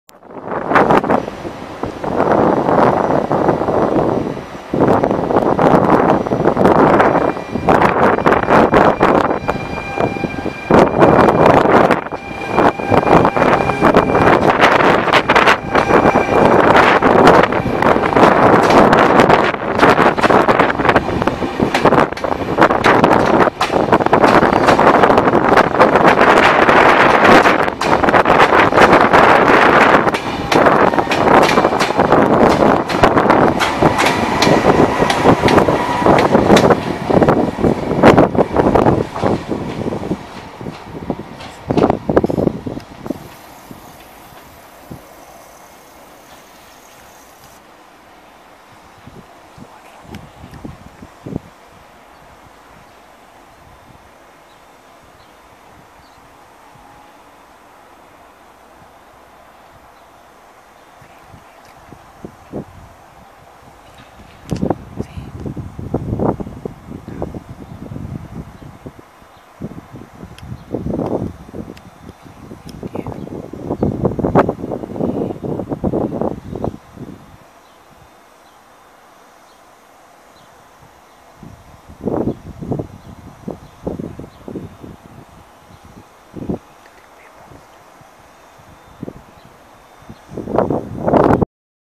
REGIONALE 11293 SAVONA-LA SPEZIA CENTRALE CON RARITA'
ECCO IL REGIONALE PER LA SPEZIA CON UNA DOPPIA TRAZIONE SIMMETRICA DI E464 E UN VIVALTO INTERCALATO!!!
E464.385 E E464.623
PARTENZA DA SAVONA, MI SCUSO PER IL VENTO MA ERA PROPRIO TANTO